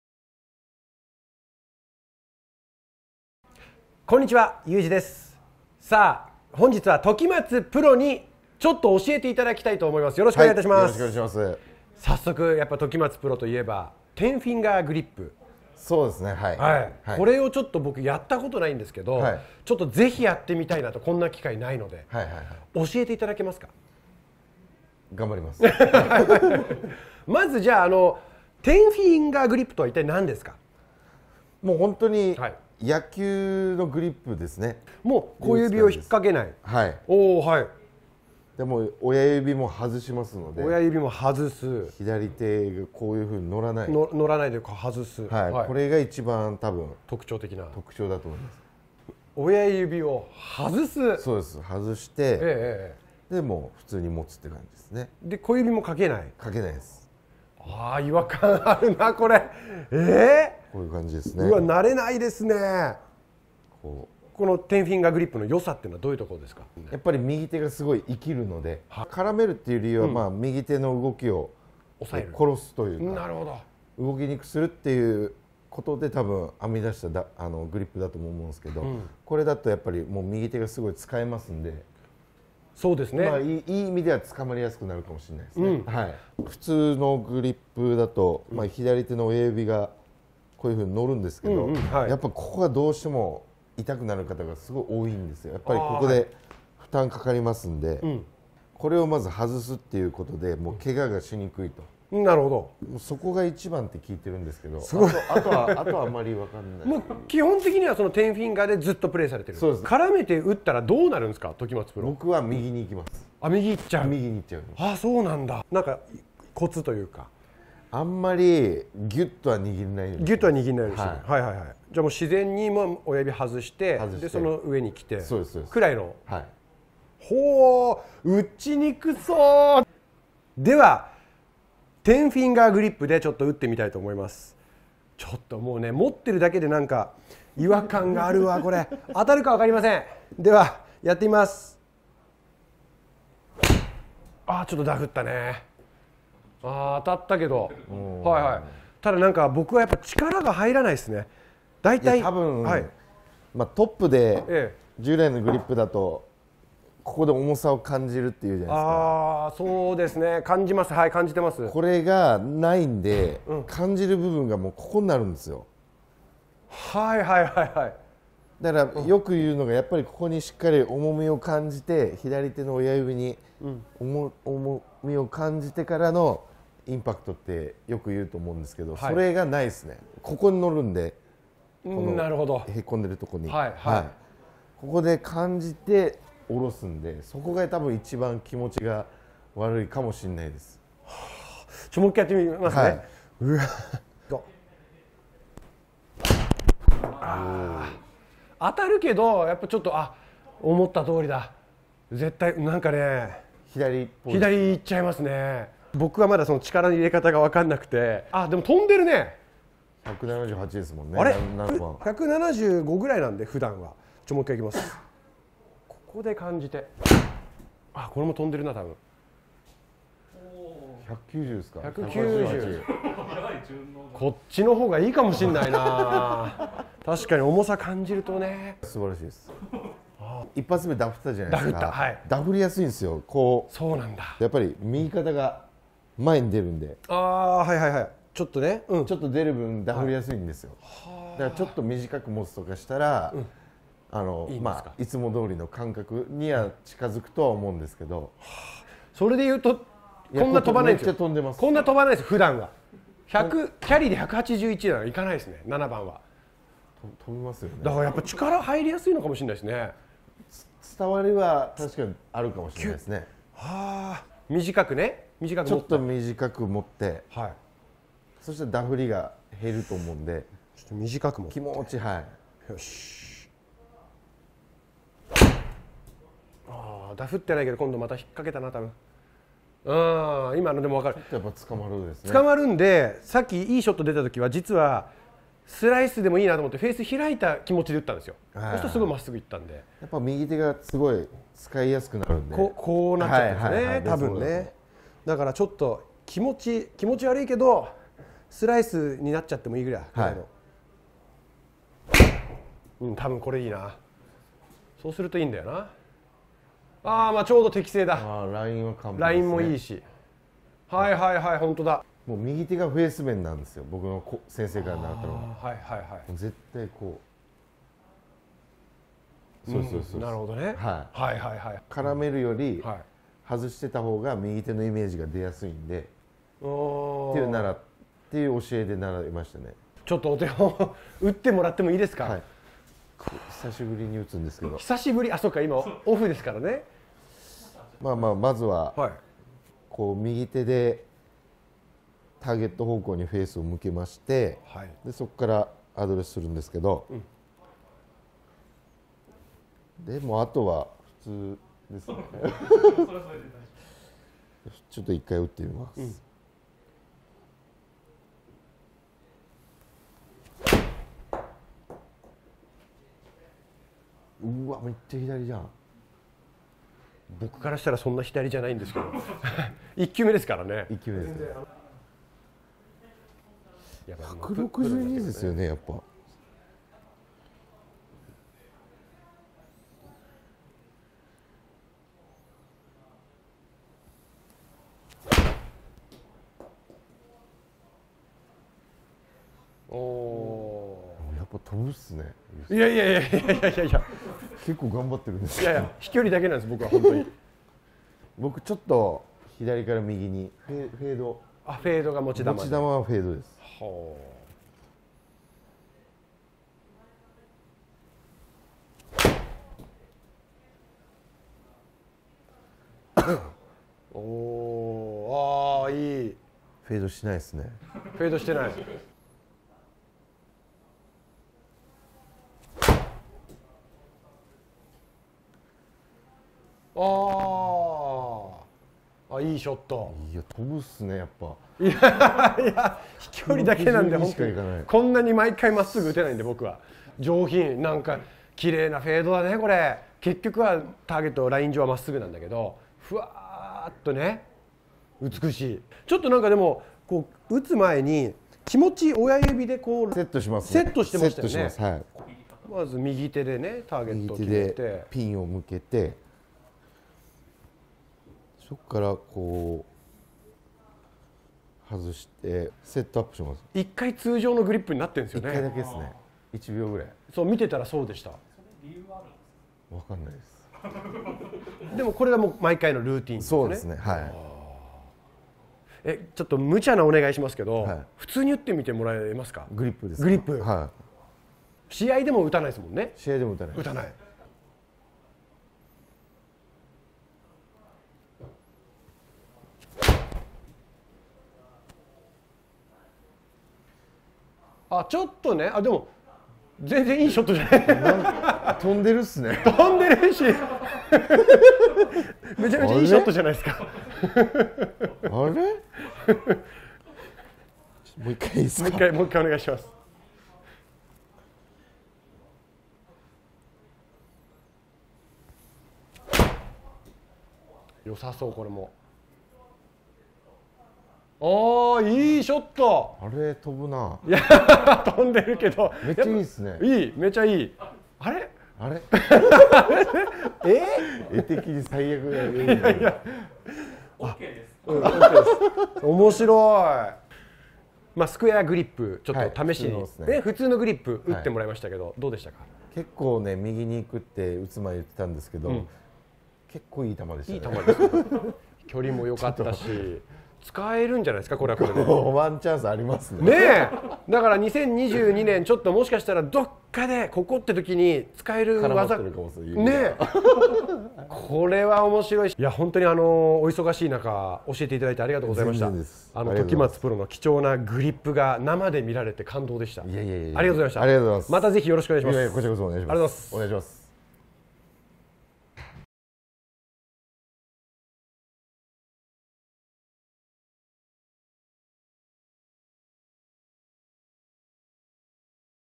【グリップ】時松隆光プロにテンフィンガーグリップを教わってみたら……スライスが激変⁉
選手会長を2年務め上げ、大の女子プロウォッチャーとしても知られる時松隆光プロにタレント・ユージが弟子入り。今回は時松プロの代名詞でもある「テンフィンガーグリップ」に挑戦してみた。驚きの結果が！？

現在進行中！
「みんなのゴルフダイジェスト」の人気企画、
ユージが有名プロたちとゴルフ上達に励む、愉快なレッスン動画はコチラをチェック↓

人気シリーズ
小澤美奈瀬×アンタッチャブル柴田 の上級者向けレッスンも見てみよう！